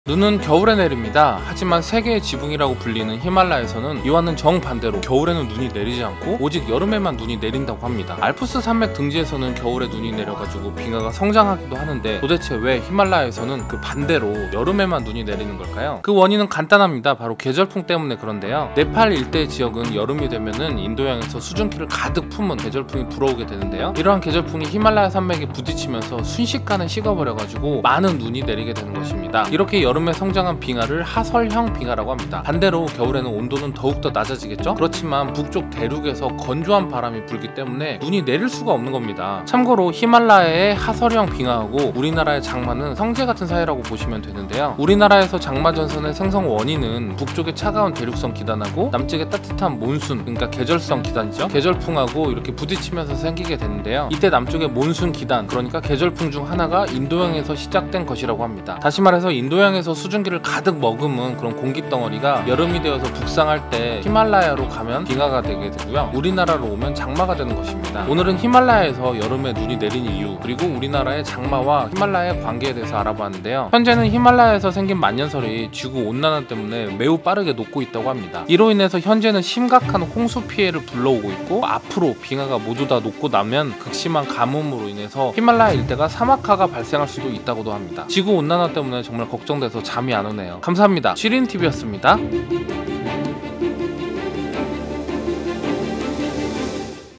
히말라야는 여름에만 눈이 내린다고?
안녕하세요 지리는 tv입니다
눈은 겨울에 내립니다.
하지만 세계의 지붕이라 불리는 히말라야에선 이와는 정반대로 겨울에는 눈이 내리지 않고 오직 여름에만 눈이 내린다고 합니다.
알프스 산맥 등지에선 겨울에 눈이 내려서 빙하가 성장한다고 하는데 도대체 왜 히말라야에선 그 반대로 여름에만 눈이 내리는 걸까요.